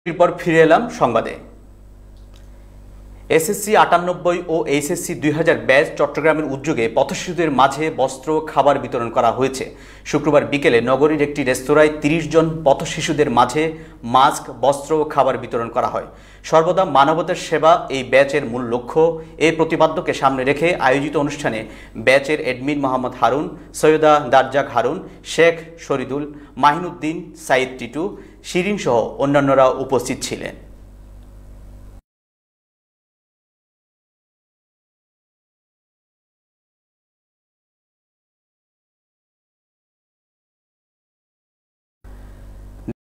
June 26, 2021
ব্যাচ ৯৮ এর উদ্দ্যেগে পথ শিশুদের মাঝে খাবার ও বস্ত্র বিতরণ।